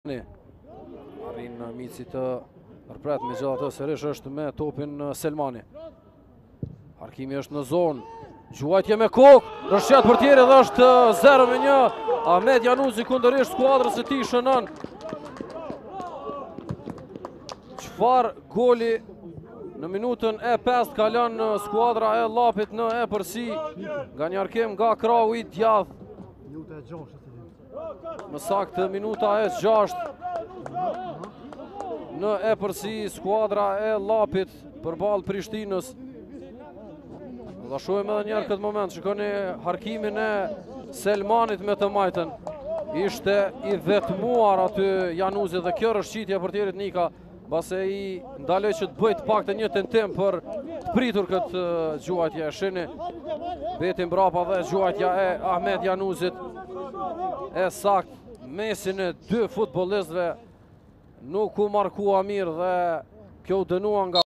Goli i pare i Ahmed Januzit ne ndeshjen Prishtina-Llapi 2:3 - 07.04.2021
Video: ArtMotion
Published by: Llapi Channel